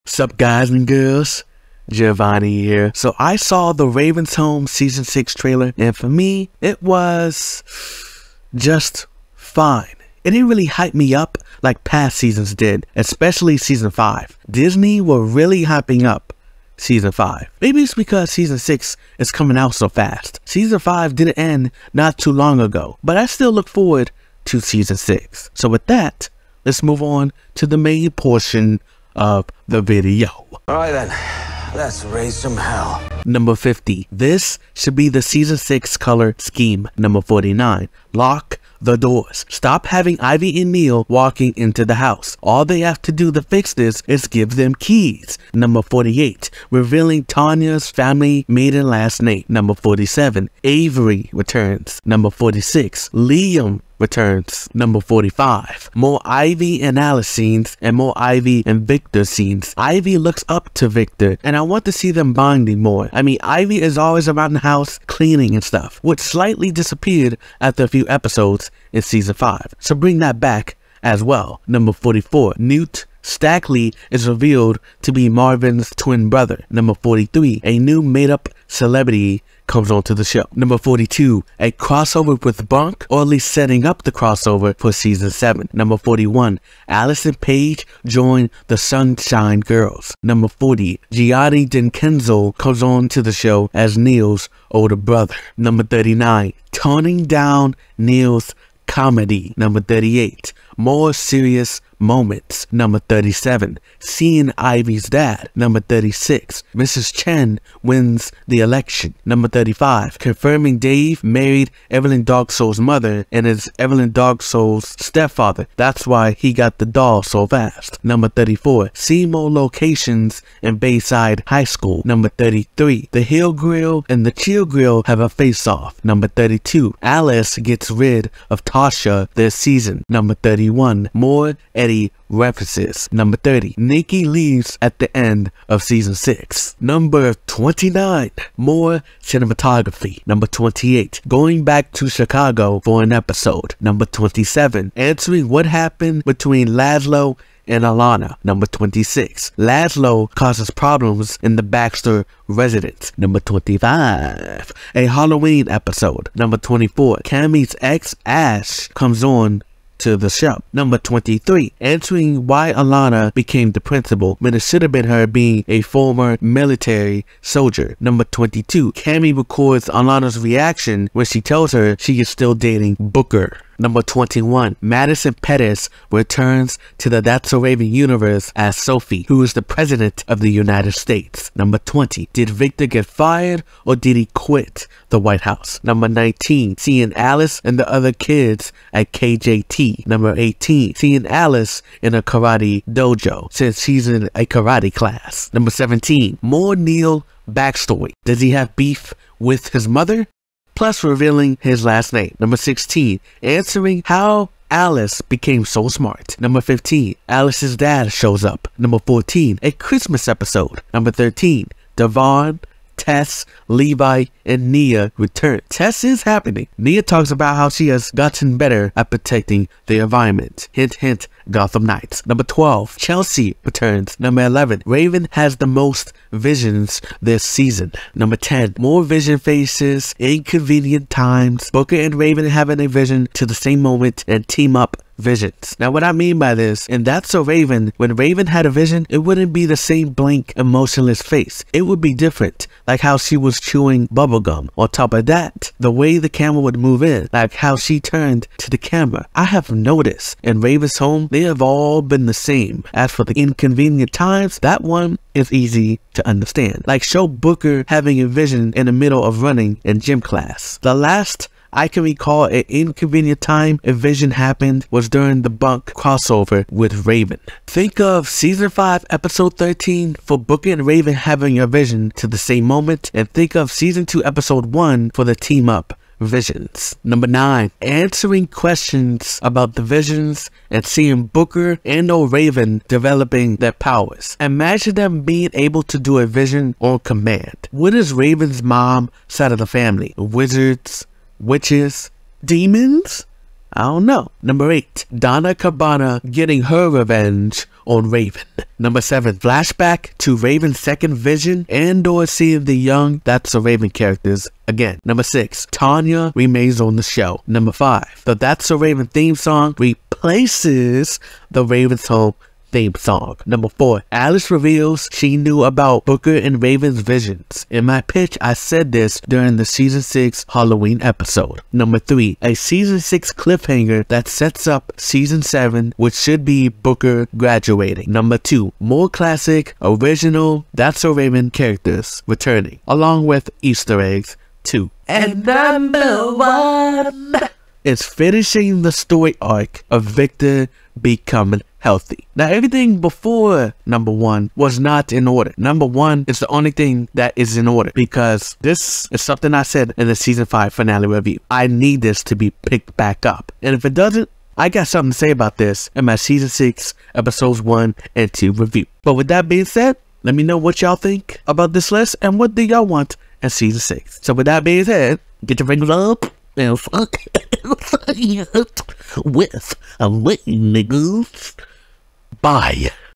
50 Things I Would Like To See Raven's Home Season 6 (Plus Thoughts On S6 Trailer)
Excatly like what I did for season 3, I give 50 things on what I predict, want, and need to see in Raven's Home season 6.

0:00 - Intro/Raven's Home Season 6 Trailer Thoughts

0:33 50-40

1:36 39-30

2:17 29-20

3:06 19-14

3:31 13-11

3:48 - 10

4:37 - 9

4:55 8-5

5:14 4-2

5:35 - 1

5:59 - Outro